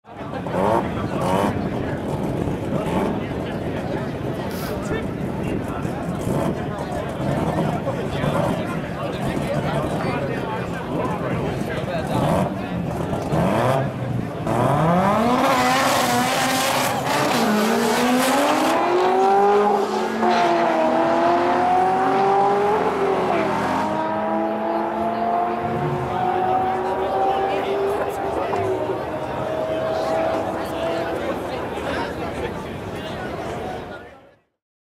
Mazda RX7 vs Z06 Corvette
This is a battle between Mazda RX7 vs Z06 Corvette Drag Race at racewaypark